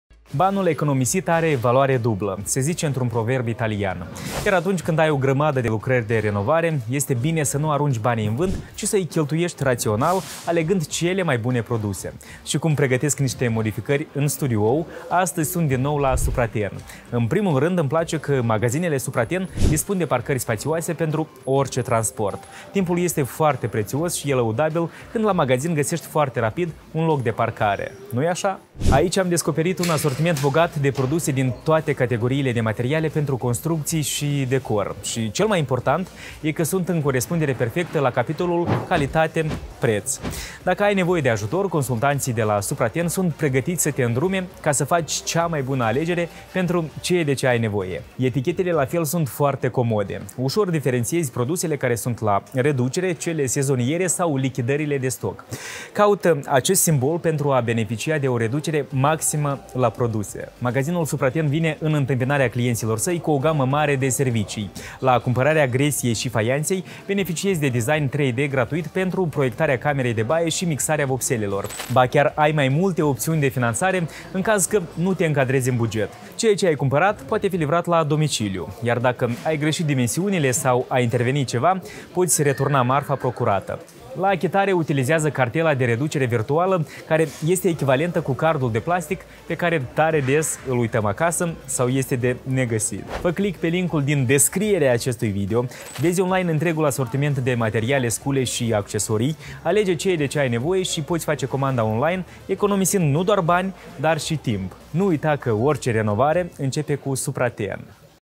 Magazinele SUPRATEN
În renovare trebuie să cheltuiești banii rațional alegând cele mai bune produse.
Cea mai bună alegere o poți face la SUPRATEN deoarece:
- găsești locuri de parcare pentru orice transport;
- alegi dintr-un asortiment bogat de produse din toate categoriile de materiale pentru construcții și decor;
- consultanții te ajută să faci cea mai bună alegere;
- etichetele de preț te îndrumă să alegi produse la care obții cea mai mare reducere;
- beneficiezi de design 3D gratuit pentru camera de baie;
- beneficiezi de serviciul mixare vopsele;
- la necesitate ai mai multe opțiuni de finanțare;
- îți cumpărăturile la domiciliu.
SUPRATEN – orice renovare începe cu noi!